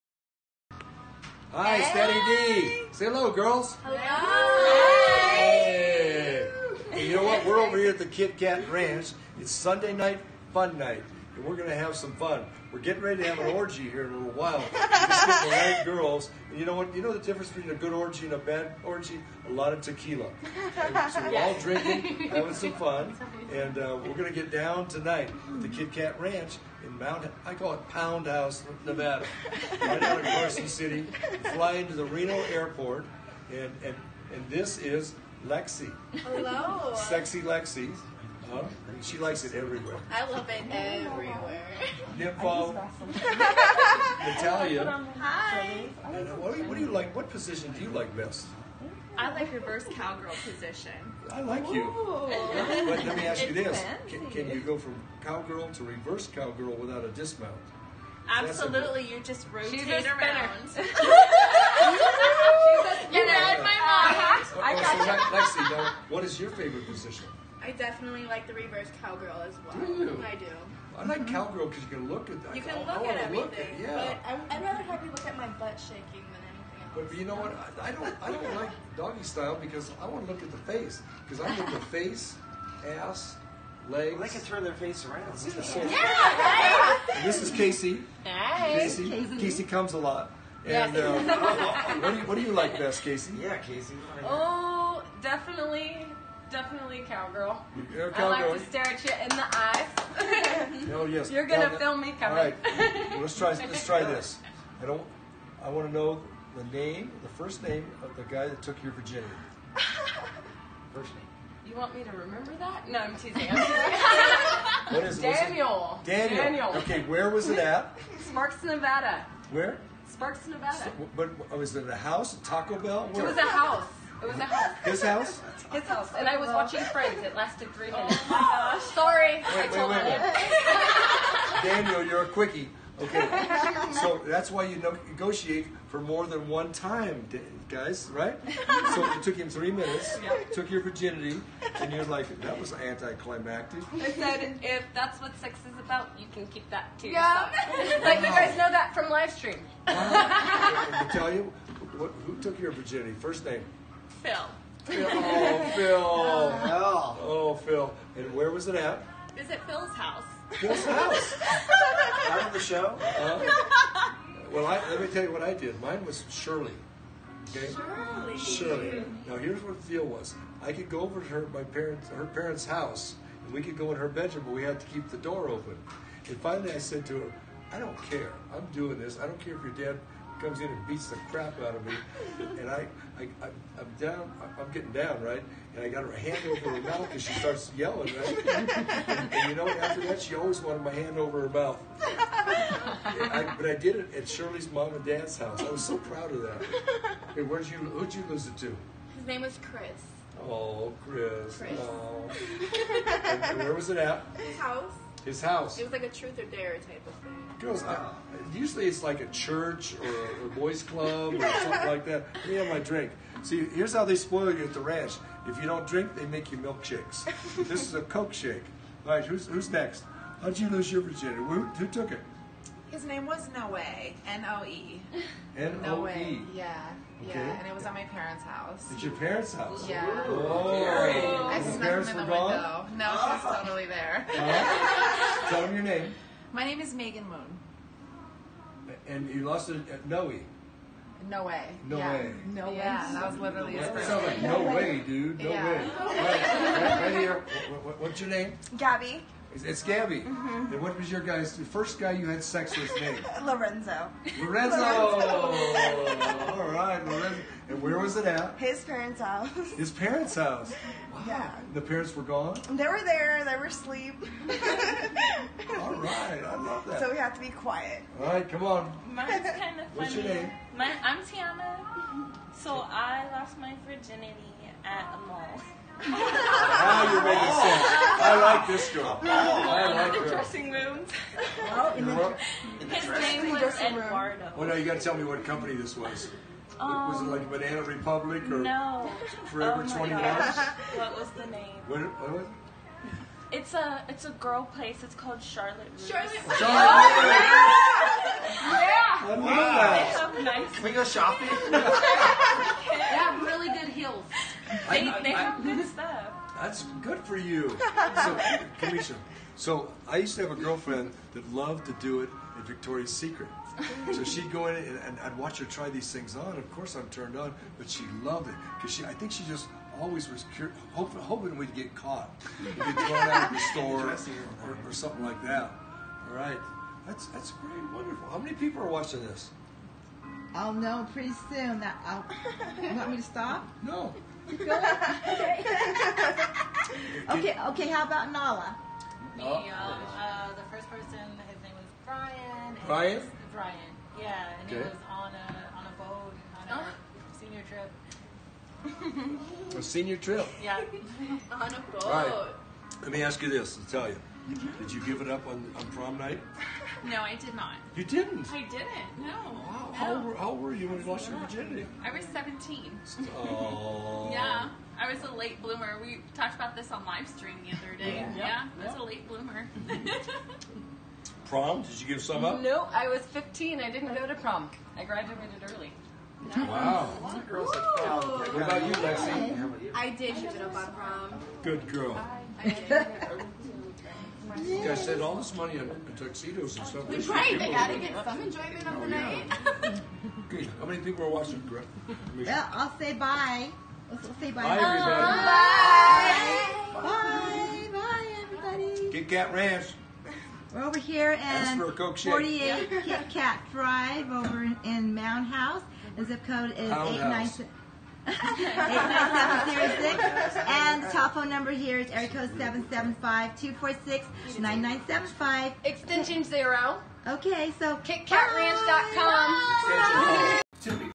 Cumisha Amado & Dennis Hof, Owner of HBO Series Moonlite Bunny Ranch At Kit Kat Ranch, Mound House,
Dennis Hof, Owner of HBO Series Moonlite Bunny Ranch At Kit Kat Ranch in Mound House, Nevada - Come and take a tour.  We have a full bar.